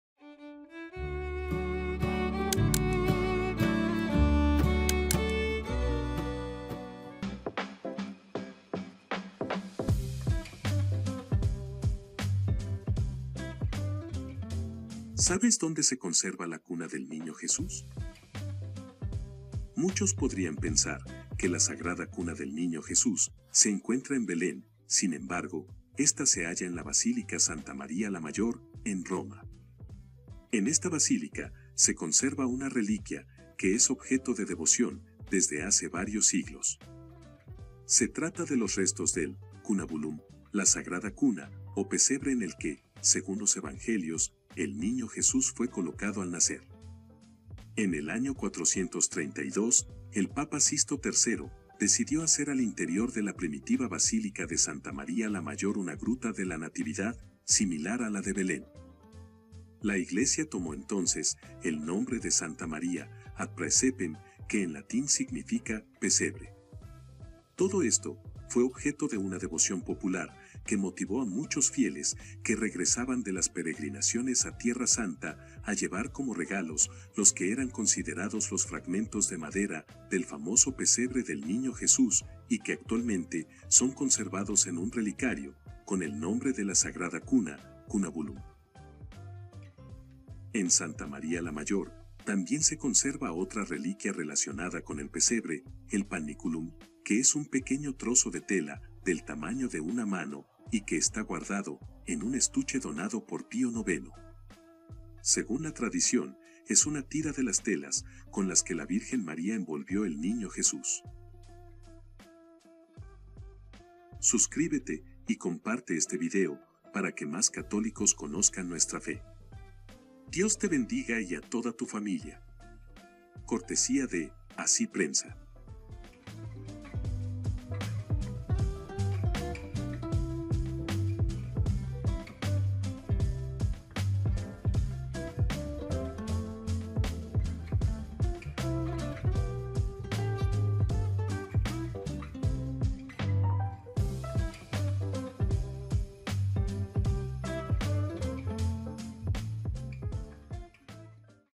SABES DONDE SE ENCUENTRAN LOS RESTOS DE LA CUNA DE JESUS ?
Esta es una gran prueba más,  de que Nuestro Señor vino a la tierra a nacer y salvar a la humanidad. Conoce tu Fe , aquí te mostraremos muchos de los tesoros que la Santa Iglesia Católica tiene para ti.